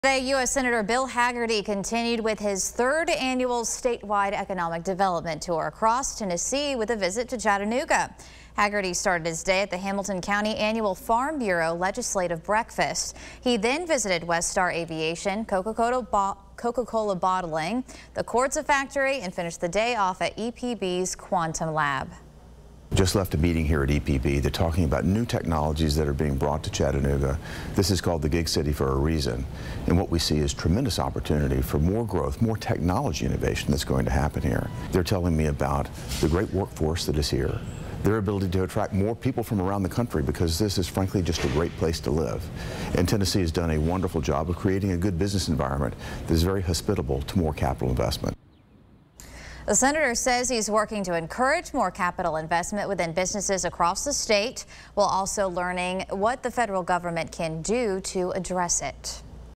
Sen. Hagerty to Continue Statewide Economic Development Tour in Chattanooga
Senator Hagerty continued his third annual statewide development tour.